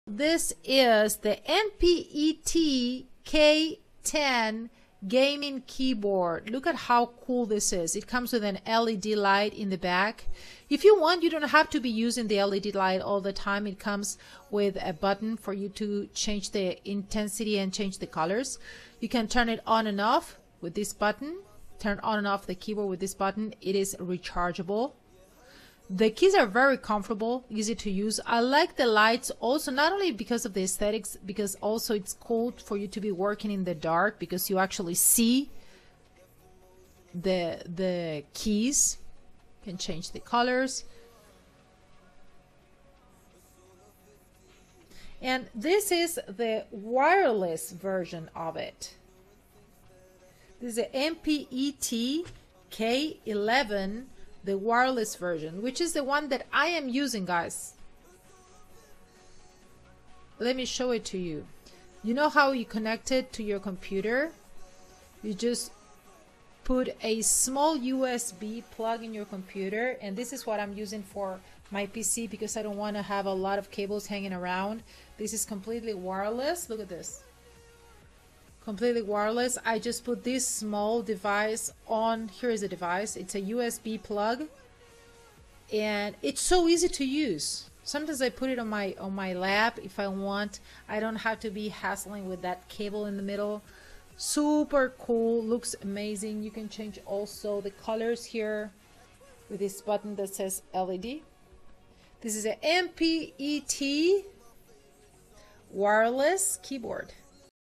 NPET K10 Gaming Keyboard Review
NPET K10 Gaming Keyboard live video review by A-List Amazon Influencer and Singer-songwriter Lorna.

NPET K10 Gaming Keyboard, LED Backlit, Spill-Resistant Design, Multimedia Keys, Quiet Silent USB Membrane Keyboard for Desktop, Computer, PC (Black)